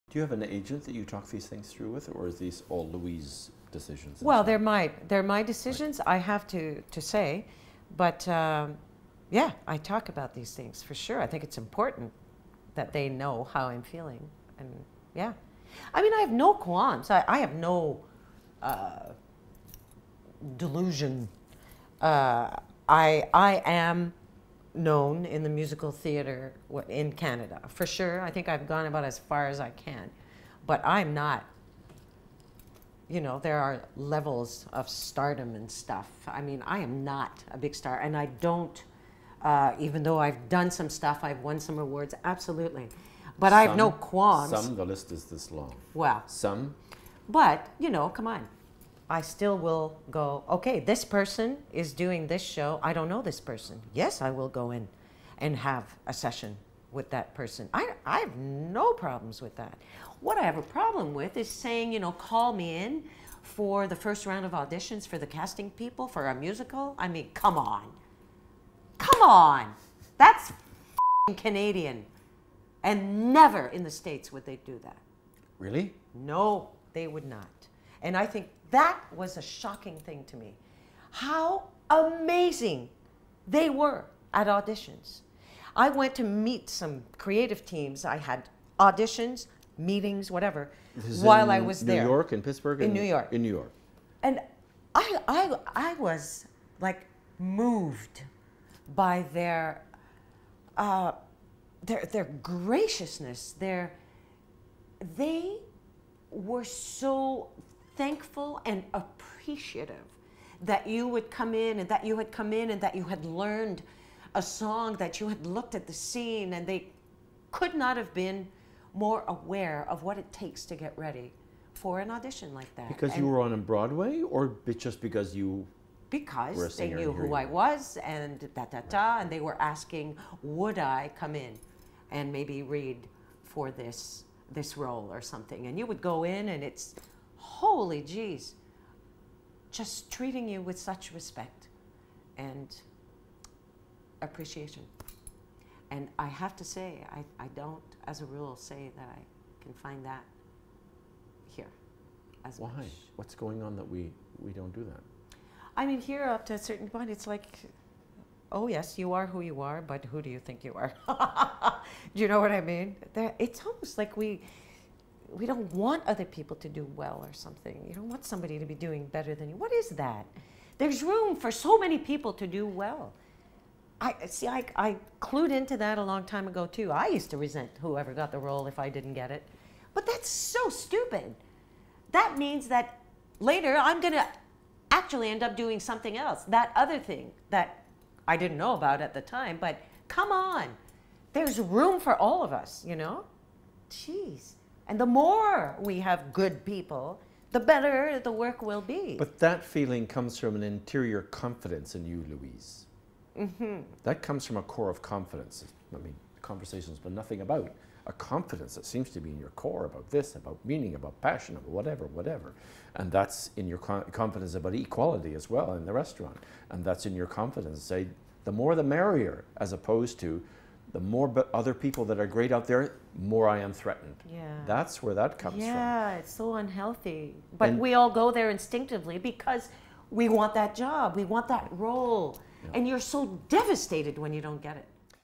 Louise Pitre on reputation, competition, and jealousy
Louise Pitre discusses the social dynamics in the competitive world of performance. (Part 9 of 19)

Interviewed by R.H. Thomson on March 27th, 2017 in Toronto.
Filming location courtesy of Dancemakers Centre for Creation.